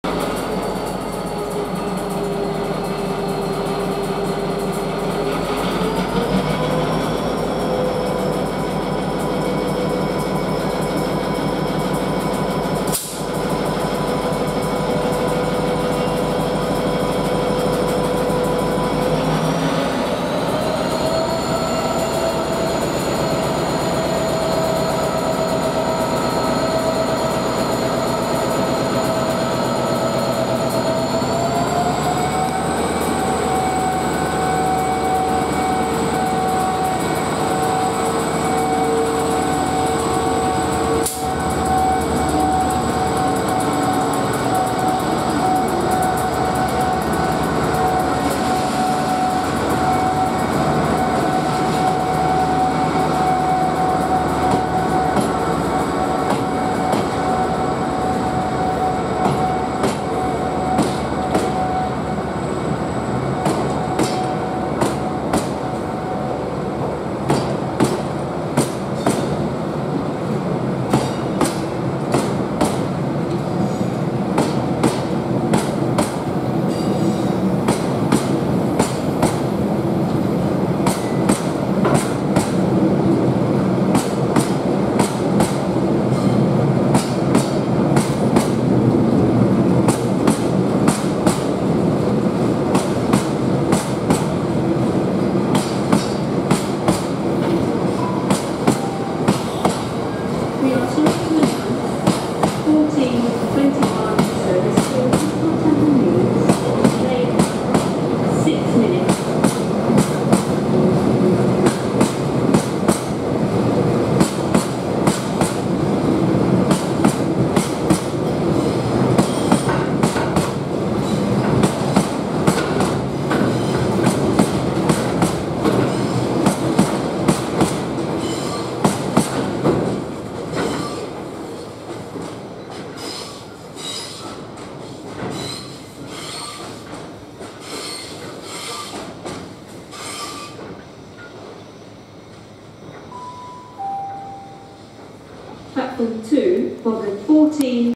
59002 with Whatley to Dagenham loaded stone train. 4/11/2015
59002 departs from Westbury with  Whatley to Dagenham loaded stone train. 4/11/2015